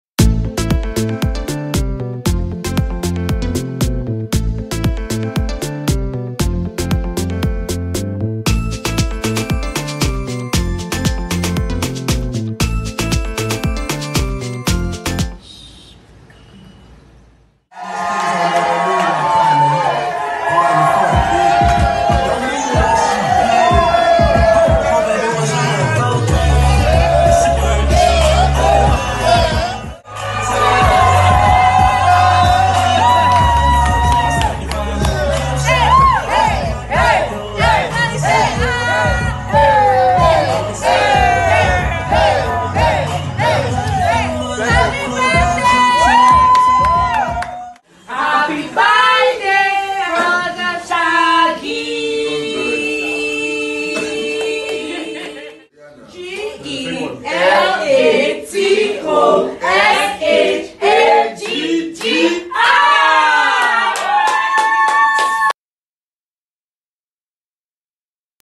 DJ Cuppy throws a birthday party ! 🎂
Dj Cuppy organized a surprise birthday party on her friend! 🎂